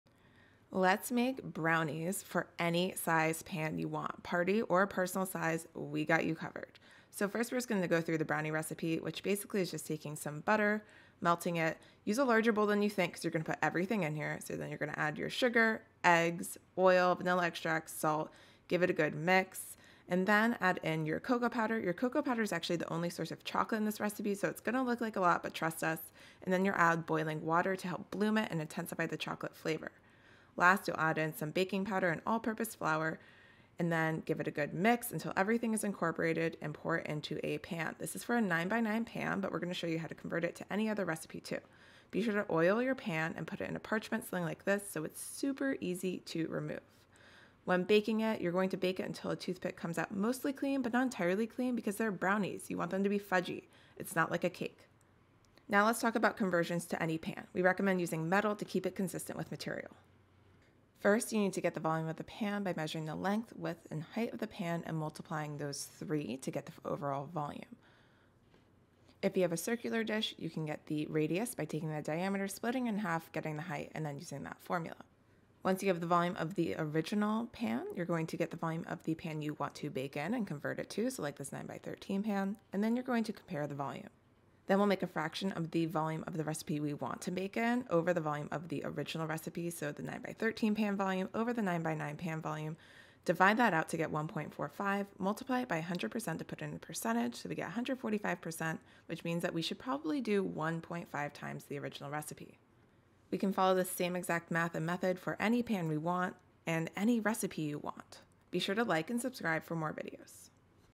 HOW TO MAKE: HOMEMADE BROWNIES FOR ANY PAN SIZE [learn math & conversions through baking]
Learn how to make the best homemade brownies. Want to learn more? Watch us explore, experiment, and explain the science behind brownies and scaling recipes in our other video

0:00-0:55 basic instructions
0:55-1:50 ways to tinker and explore